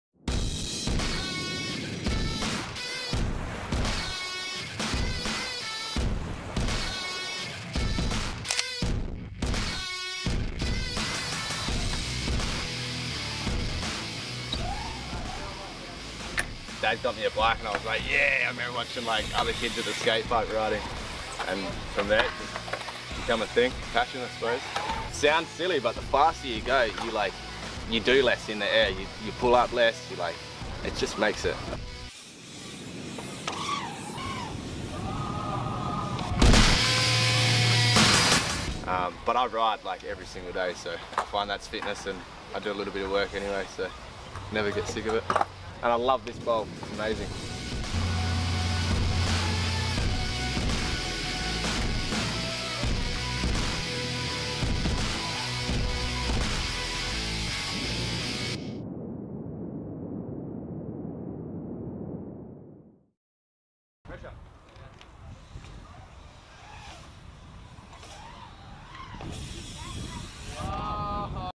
Busselton Skate Park with Pro BMX rider Shane Ellis
Recorded on Sunday at the Skate Park in Busselton.
Shane Ellis, 28 years old, is a Professional BMX Athlete from Australia (WA).
Follow him on Instagram @_shaneellis_

GEAR △ Camera: Canon EOS M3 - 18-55mm lens // Mic: Rode VideoMic // Drone: DJI Mavic Mini